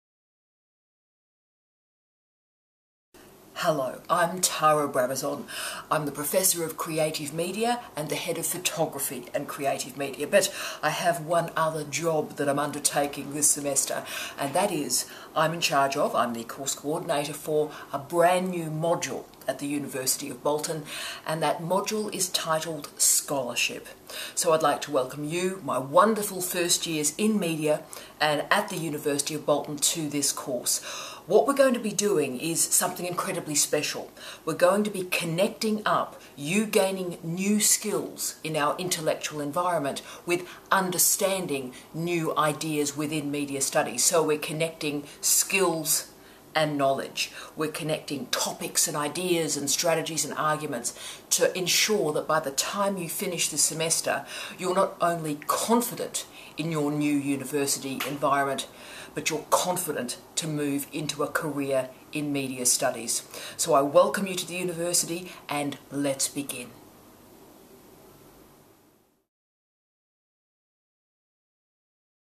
Welcome to Scholarship Professor Tara Brabazon
Tara welcomes her new first year students to "Scholarship" - a brand new course at the University of Bolton